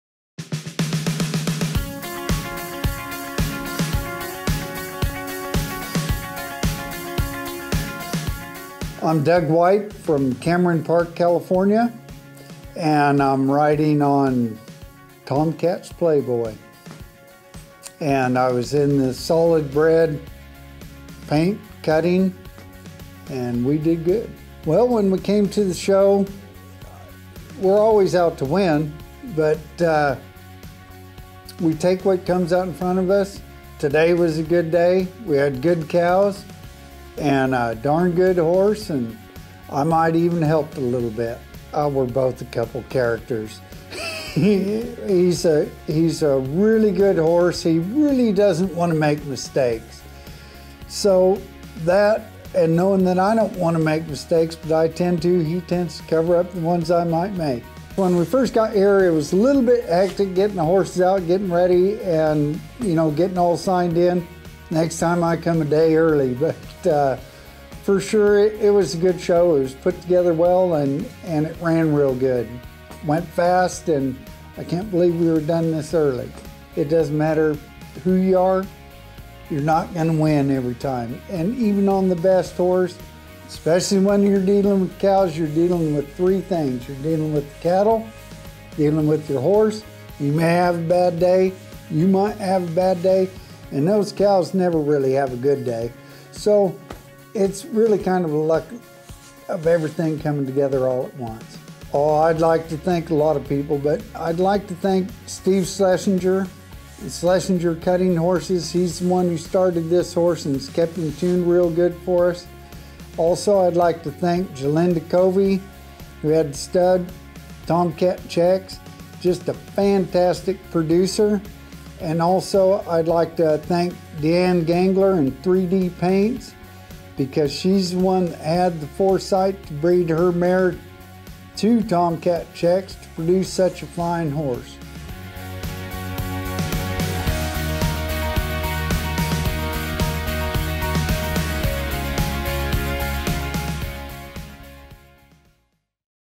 Winning Words - Doug White (APHA World Championship Show 2014)
Doug White talks about his winning ride on Tomcats Playboy in Solid Paint-Bred Cutting at the 2014 APHA World Championship Show.